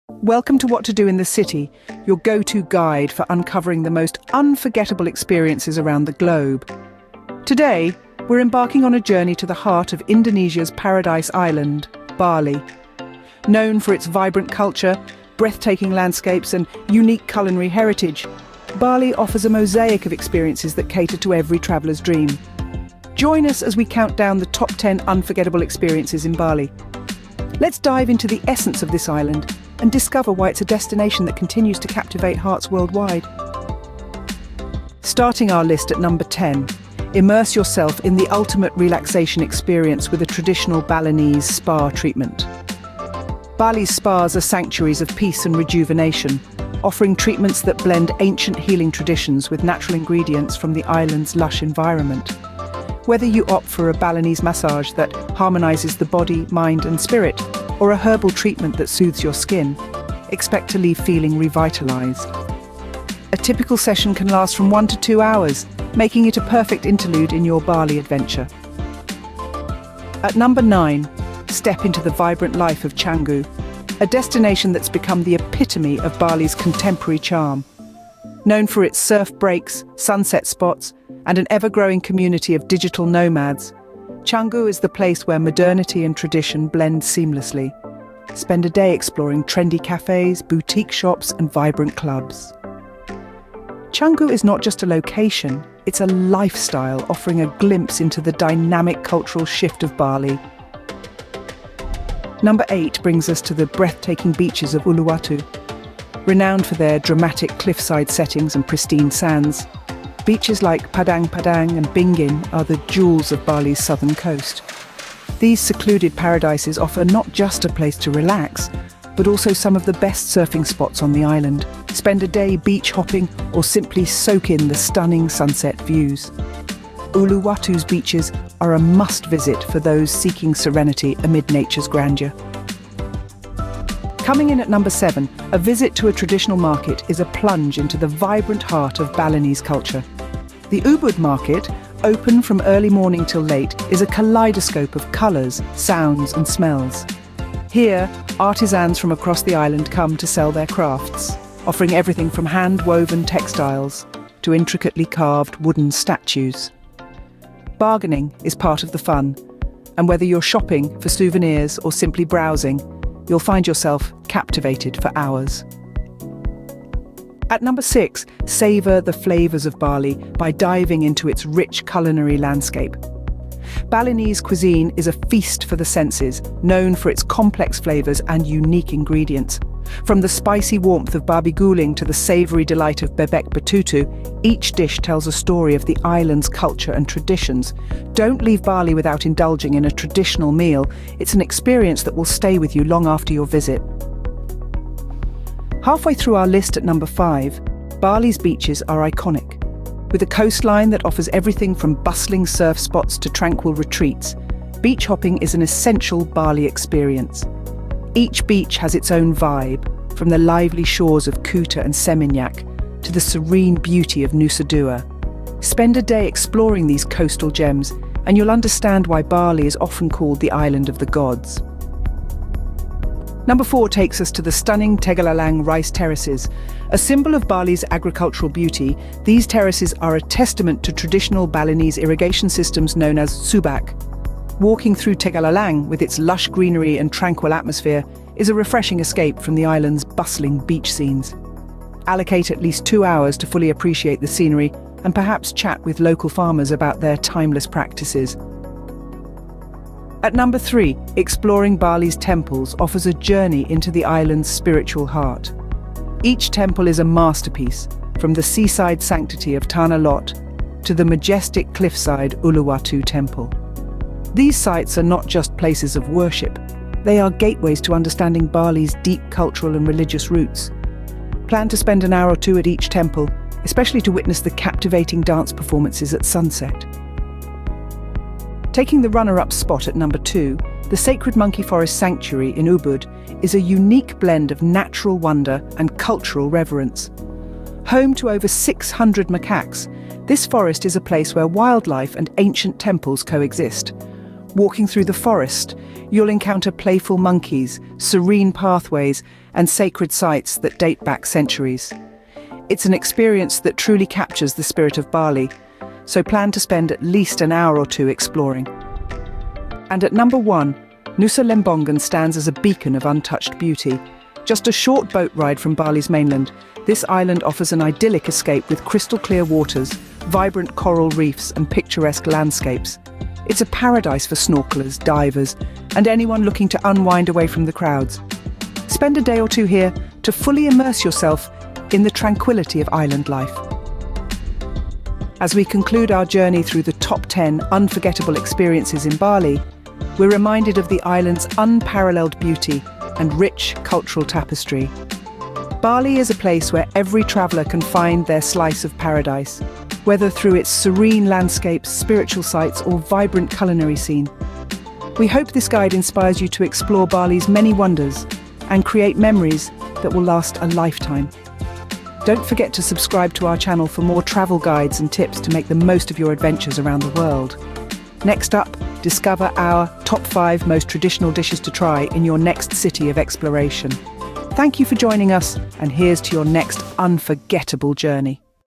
What to do as a Digital Nomad in Bali | 10 Must-Dos
Planning on becoming a digital nomad in Bali? In this video, we're taking you on a virtual tour of what Bali has to offer. From exploring pristine beaches to immersing in cultural experiences, we've curated a list of activities that will make your Bali trip unforgettable. Whether you're a beach lover, foodie, or adventurer, Bali has something for everyone!

Join us as we highlight the top attractions and hidden gems that should definitely be on your itinerary when visiting Bali. Get ready to be inspired and start planning your dream vacation to this tropical paradise. Let's spread the wanderlust together!

00:00:00 Welcome to Bali
00:00:34 Relax with a Balinese Spa Treatment
00:01:10 The Busy Canggu
00:01:42 Uluwatu's Beaches
00:02:14 Visit a Traditional Market
00:02:47 Try Balinese Cuisine
00:03:15 Beach Hopping
00:03:43 Tegalalang Rice Terraces
00:04:10 Explore Bali’s Temples
00:04:38 The Sacred Monkey Forest Sanctuary
00:05:09 Nusa Lembongan
00:05:35 Conclusion